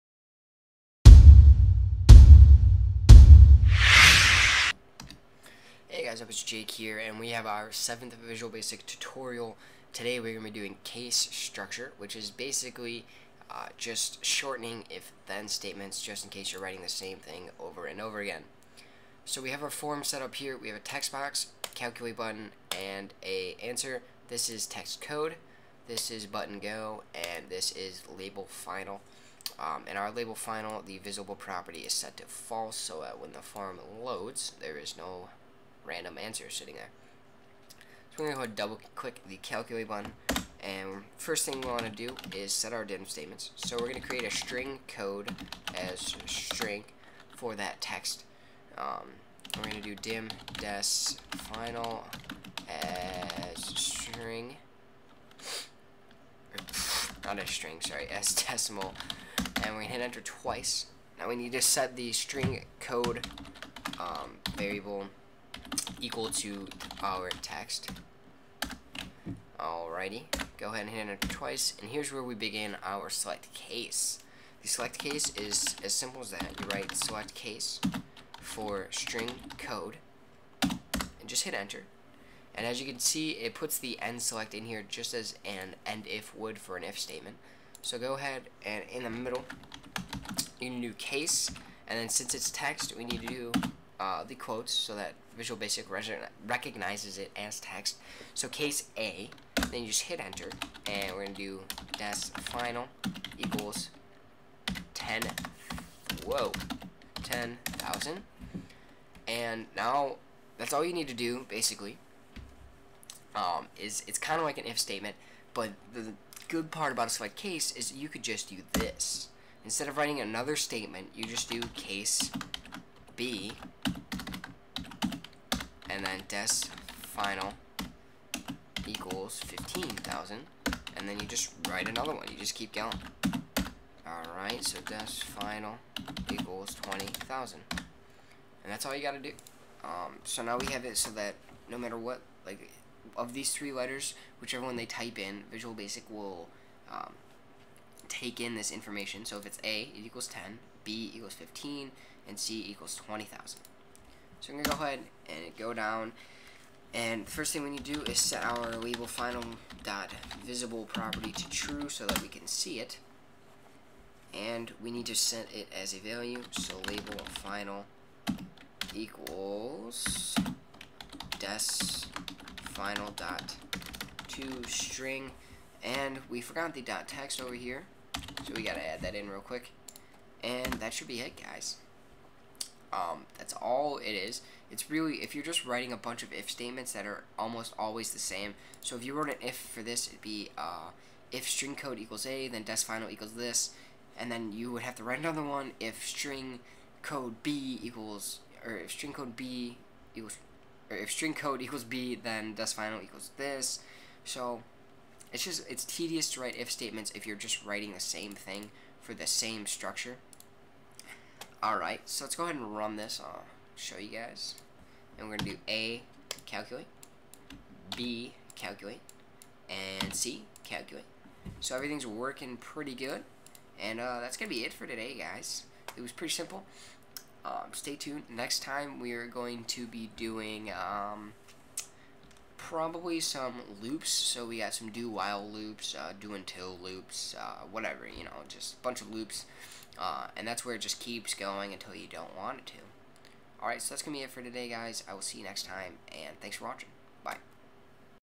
Visual Basic Programming Tutorial #7: Select Case
Waddup, today's tutorial is on select case statements. They are a pretty simple and faster way for you to creat mulitiple outcomes for variables. I hope you guys enjoy. Next time we will make some loops!